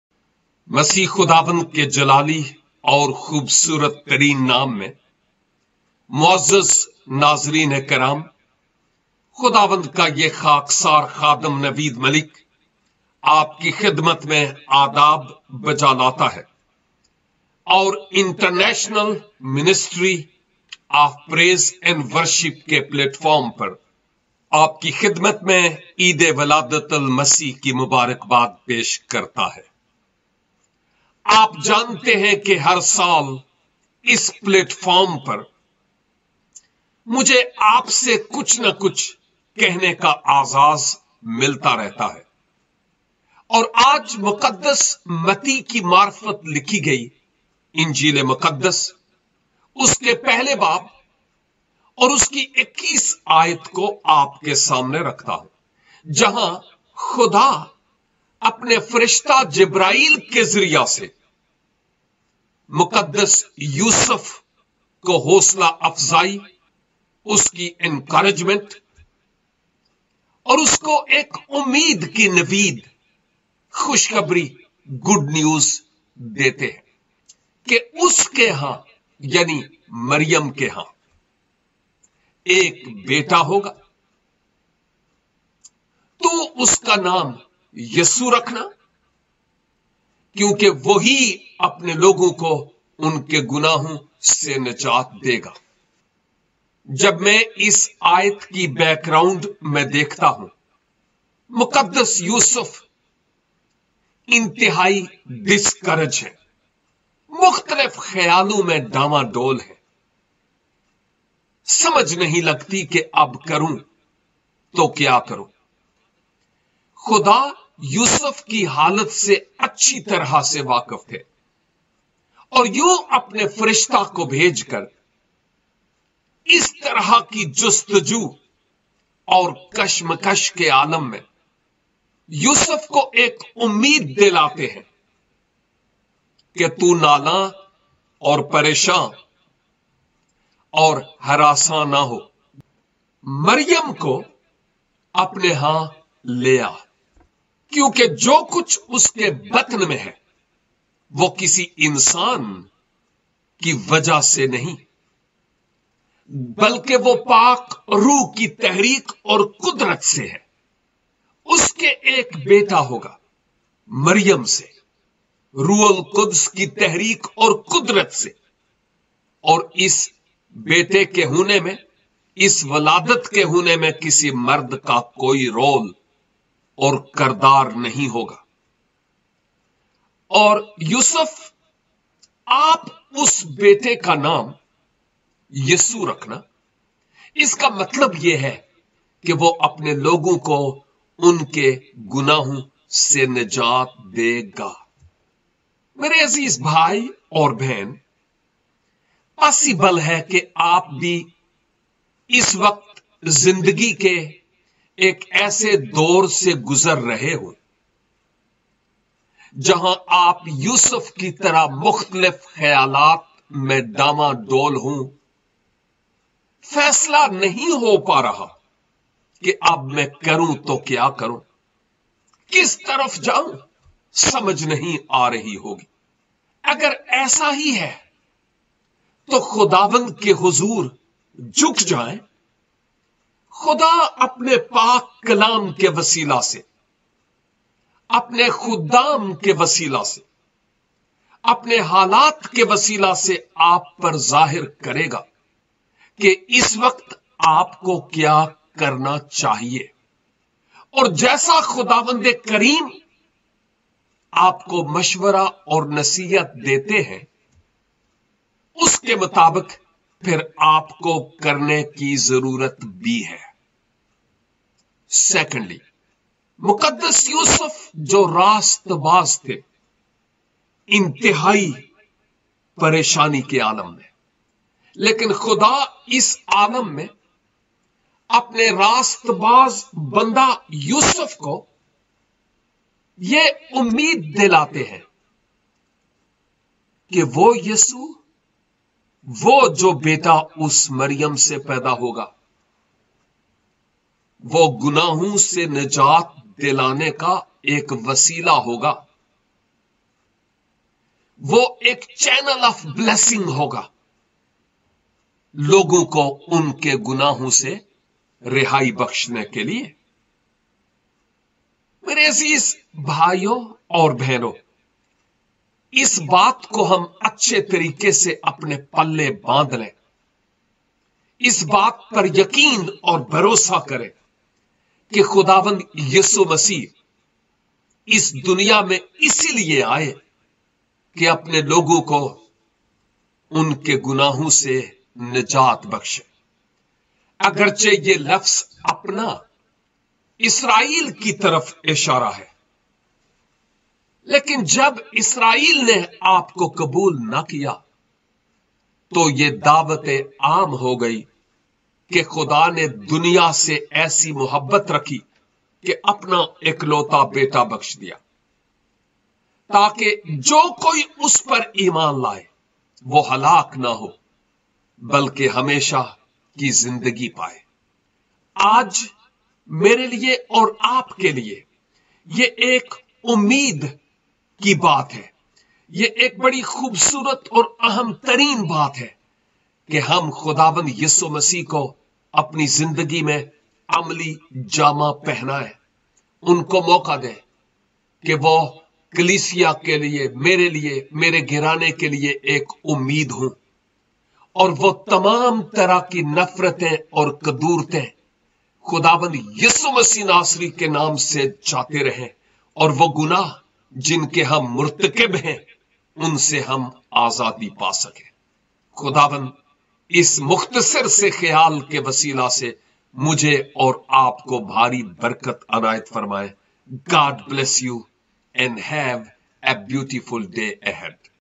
Hope in Difficult Circumstances Urdu/ Hindi
Hope in difficult circumstances for a believer. So short but very meaningful video to watch. Be blessed in Jesus name.